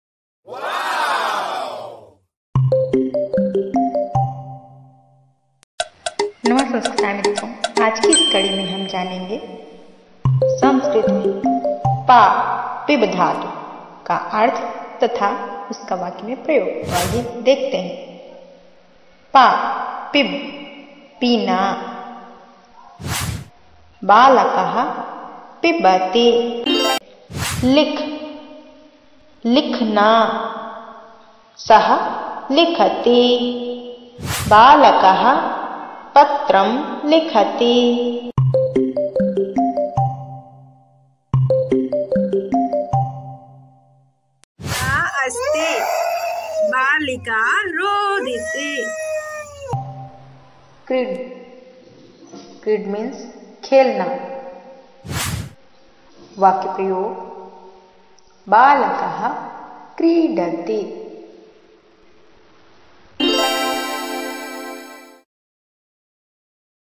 Sanskrit ki kuchh dhatuon ke arth /संस्कृत की कुछ धातुओं के अर्थ तथा वाक्य प्रयोग
इस वीडियो में आप सभी को संस्कृत की कुछ धातुओं के अर्थ तथा वाक्य प्रयोग संस्कृत में अत्यंत सरलता से चित्रों के माध्यम से स्पष्ट किया गया है। जो आप सभी के लिए अत्यधिक महत्वपूर्ण है।